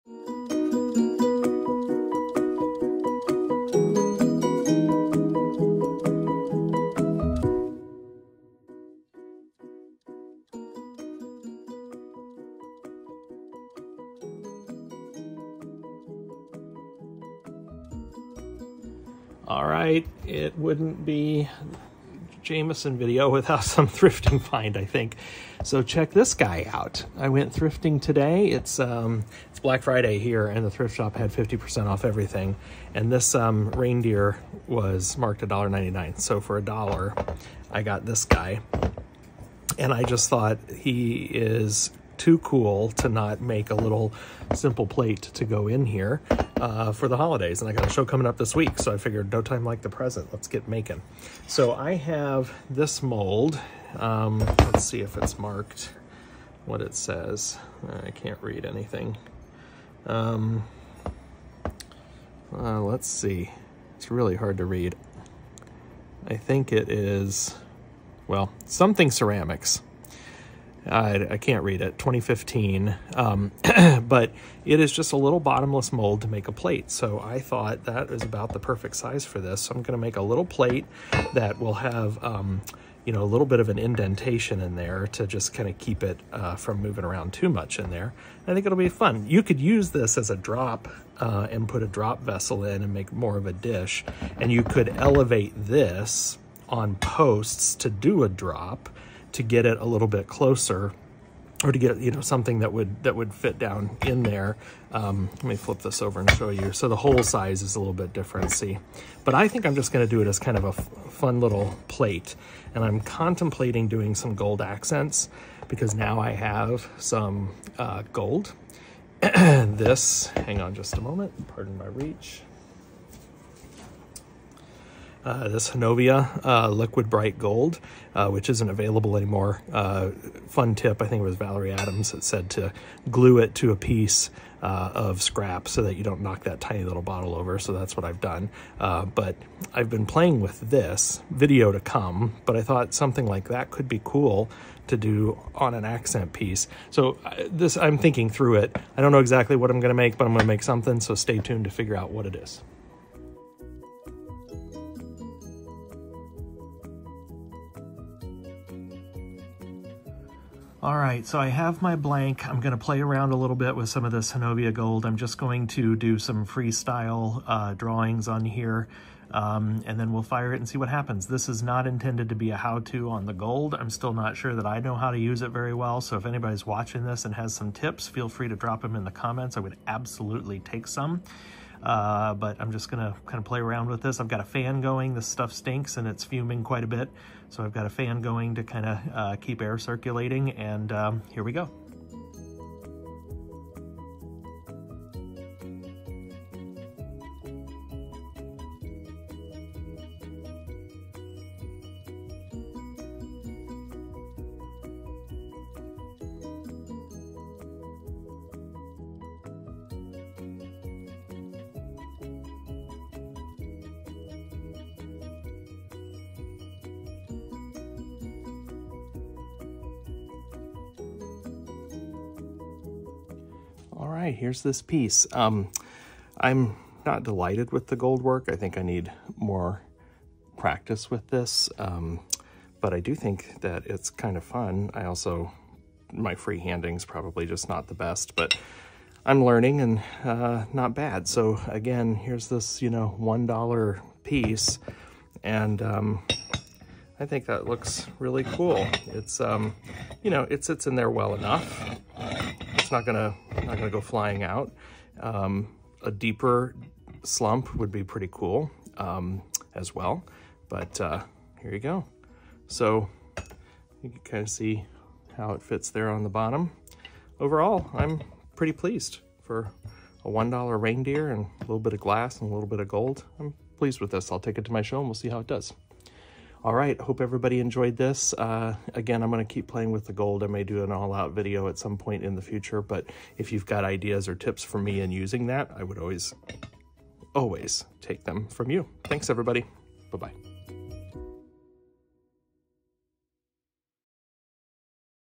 I Always Love Upcycling a ThriftFind, Especially When it Turns Out Like This!
Reindeer: thrifted
Gold luster: (no longer sold)
Turntable: I started an Amazon store featuring my favorite glass tools and supplies.

The first firing of the plate blank was a standard full fuse firing.

Then the gold was fired on and the plate was slumped in the same firing with this schedule:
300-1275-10 (kiln vented up to 1000)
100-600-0